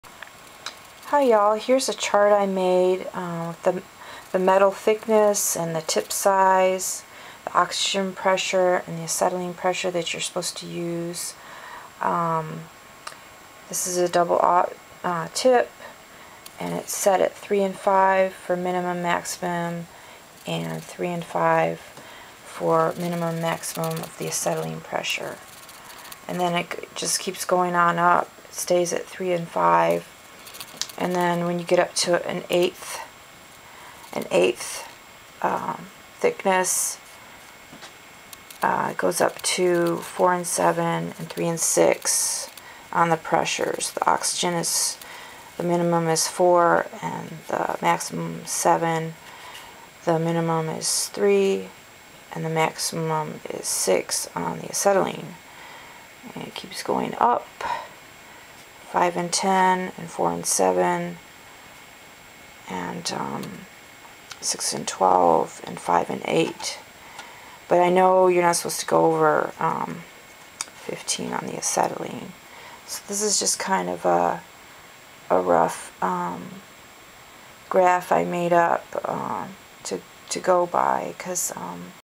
METALTHICKNESS AND PRESSURES JAN 25, 2011 001.AVI
Welding Chart: This is the graph chart of the metal thickness, tip size, oxygen and acetylene pressures that are listed in the book.  It says to also never go over 15 on your acetylene max. pressure